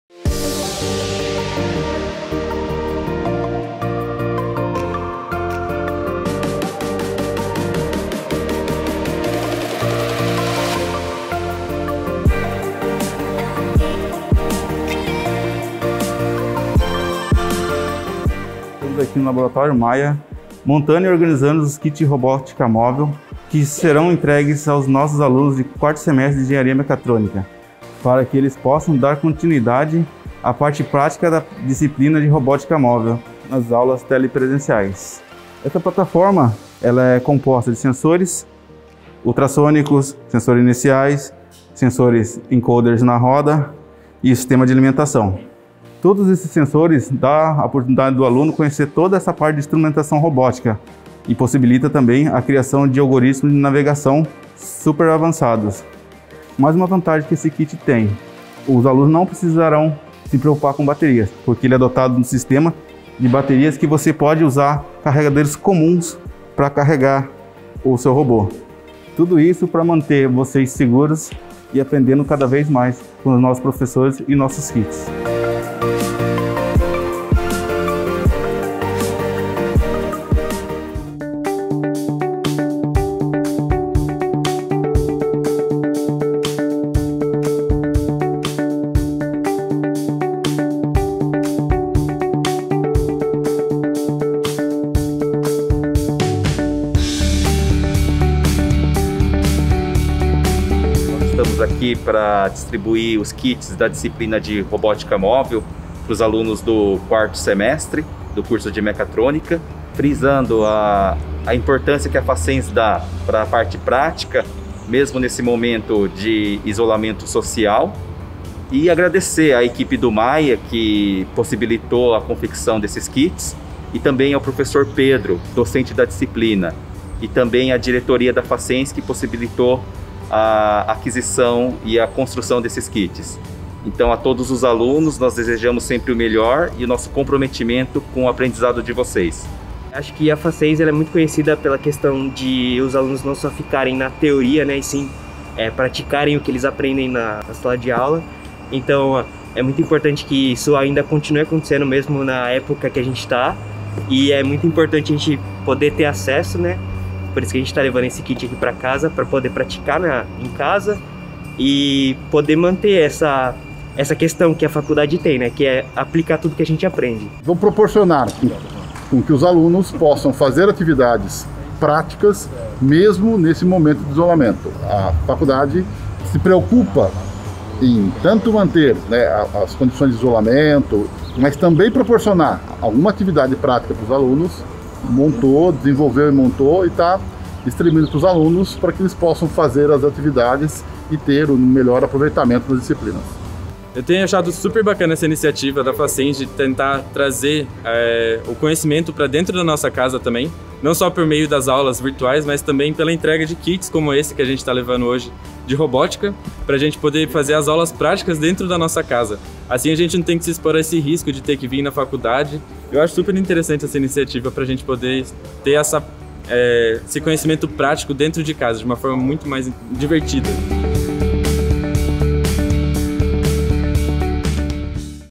Engenharia Mecatrônica Facens - Entrega dos Kits Práticos de Robótica
A fim de levar o hands on da Facens para a casa dos alunos, a coordenação e professores da Engenharia Mecatrônica do Centro Universitário realizaram a entrega de Kits práticos para os alunos do curso.
Esse kit de robótica, construído em parceria com o MAIA Facens (Mobilidade Aplicada e Inteligência Artificial) e o FabLab Facens, permite que os estudantes tenham experiência do mão na massa em suas casas e possam dar continuidade ao conteúdo prático do curso, mesmo no período de distanciamento social.